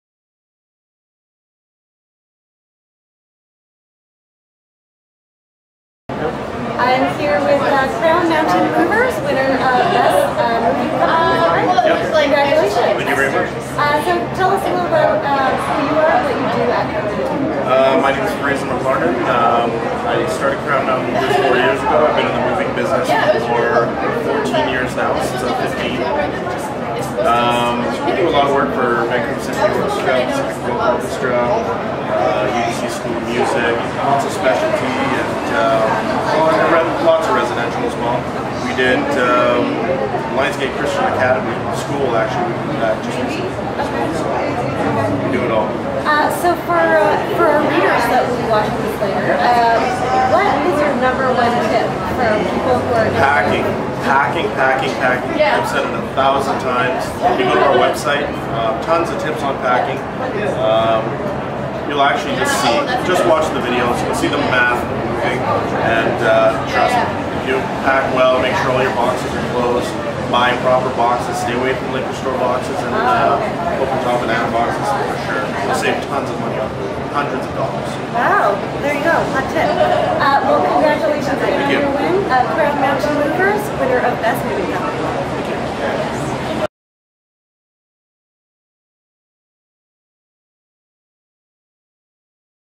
Crown Mountain Movers at Best of Vancouver 2014
Crown Mountain Movers, winner of Best Local Moving and Storage Company at the Georgia Straight's 19th annual Best of Vancouver awards.